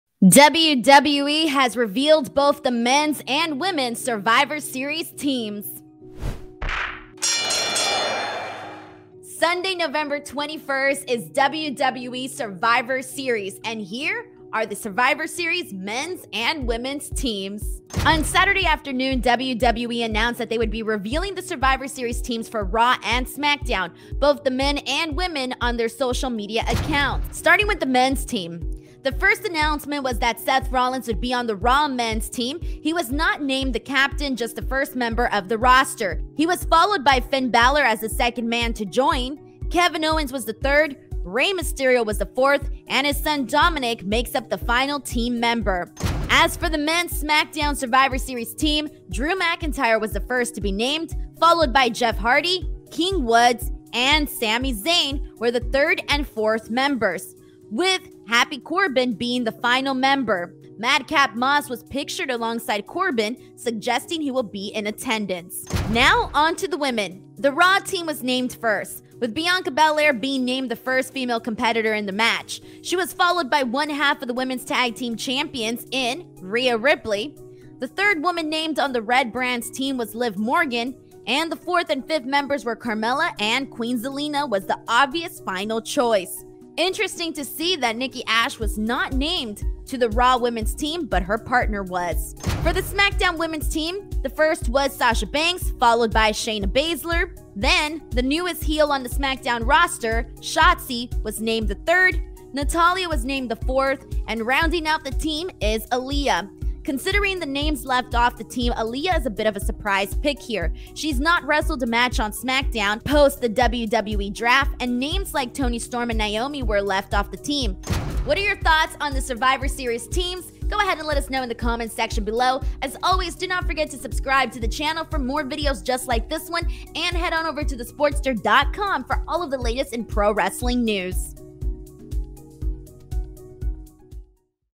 WWE Reveals 2021 Survivor Series Teams
WWE has also revealed both the men’s and women's Survivor Series teams.